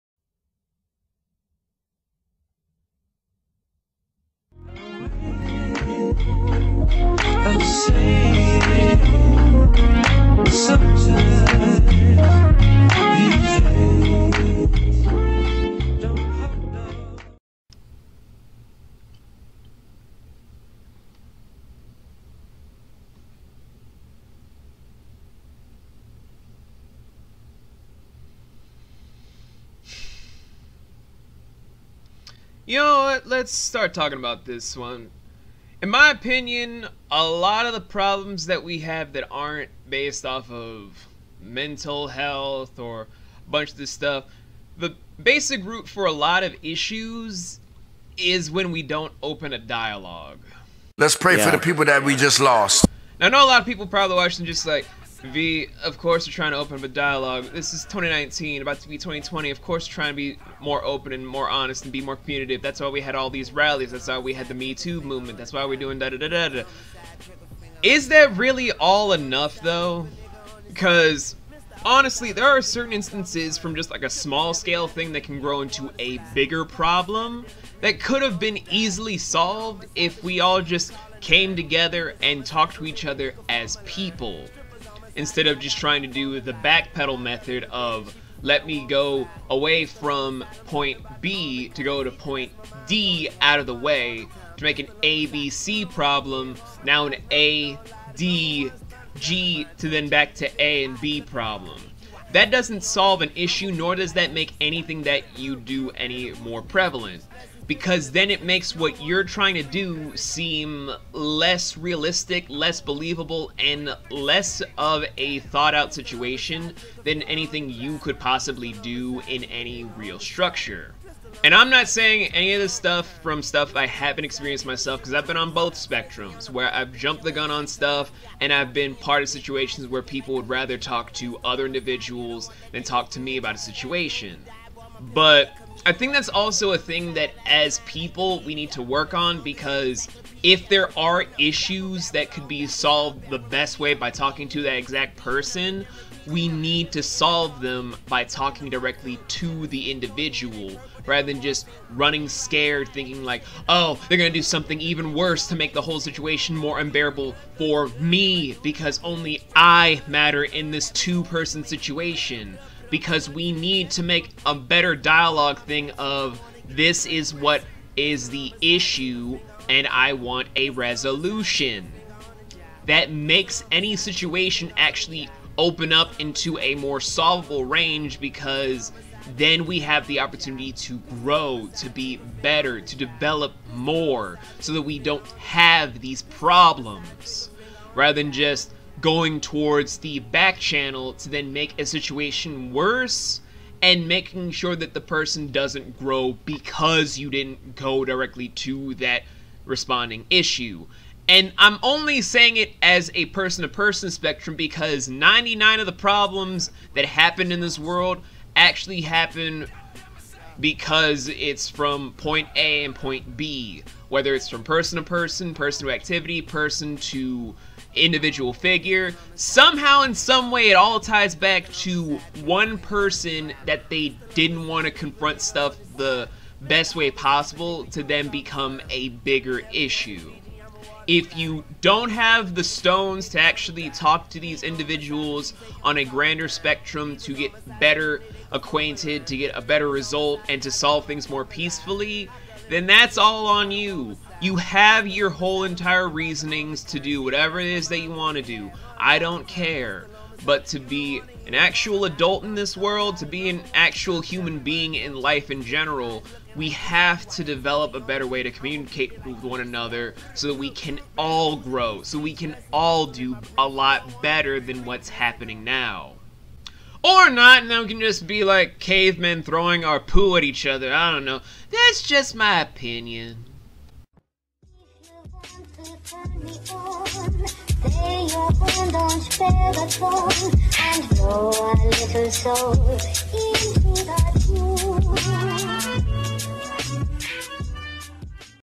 Nomadic Opinions S2: Communication Is A Must || The Pyramid (N)
In this installment of "Nomadic Opinions", we return to tackle a topic that is really needed in today's society - Failed Communication & Why We Need To Do Better.
Let's begin.